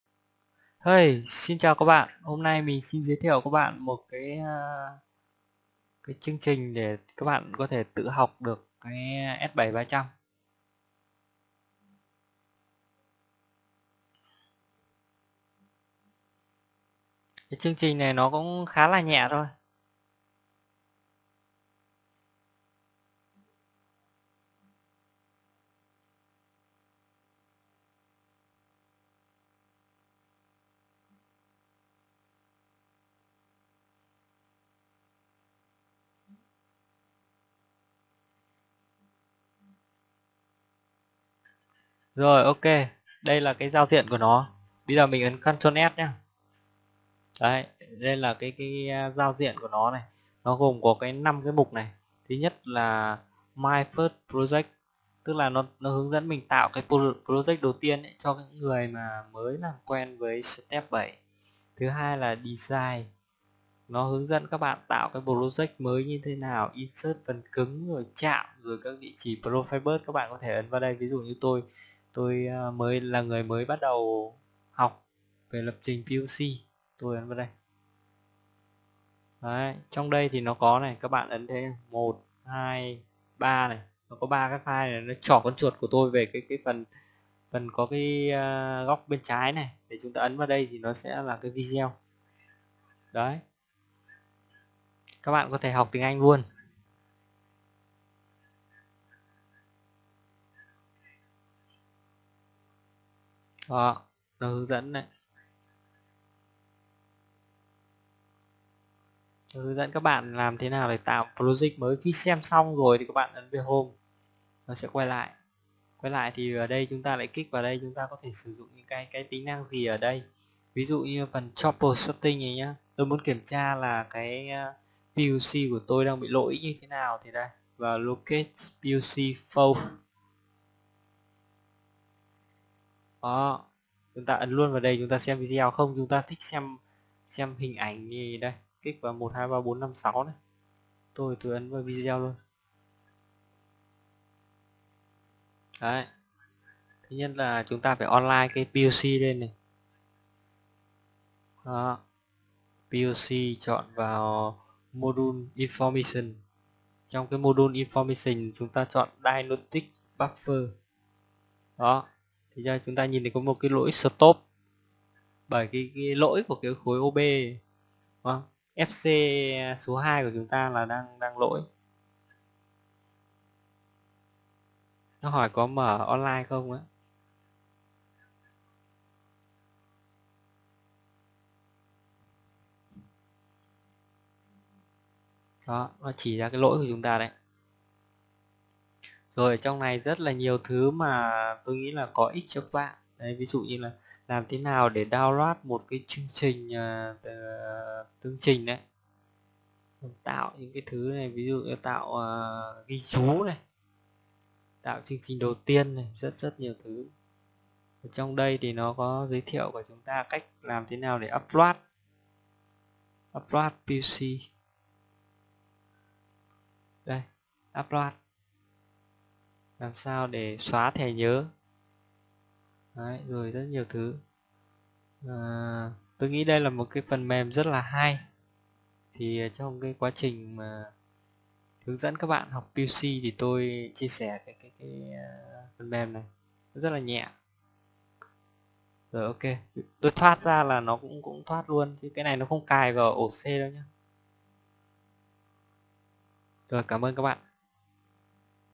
Phần mềm tự học PLC S7 - 300/400
Phần mềm giúp bạn có thể tự học Plc S7 - 300/400 hiệu quả nhất